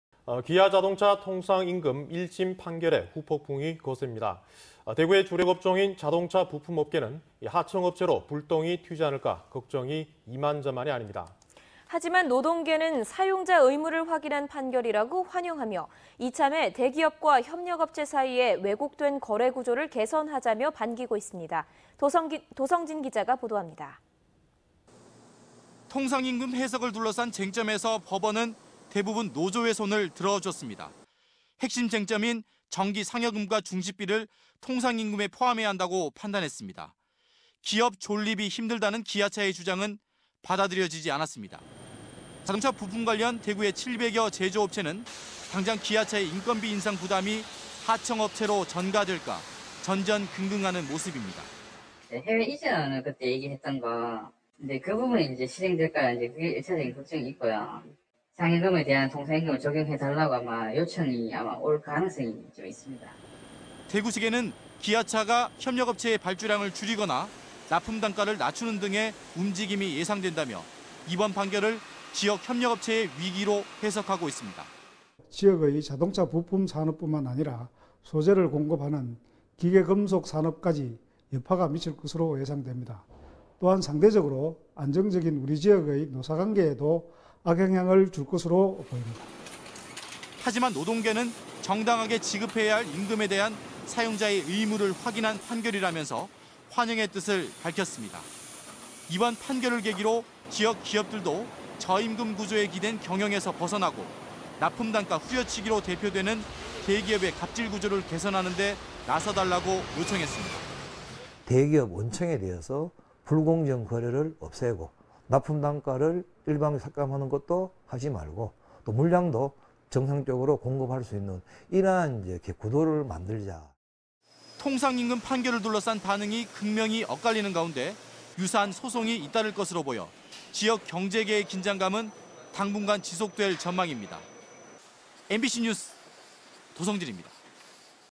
[대구MBC뉴스] 기아차 통상임금 판결...지역 부품업계 긴장
내용: 기아차 통상임금 판결...지역 부품업계 긴장 - 도성진 기자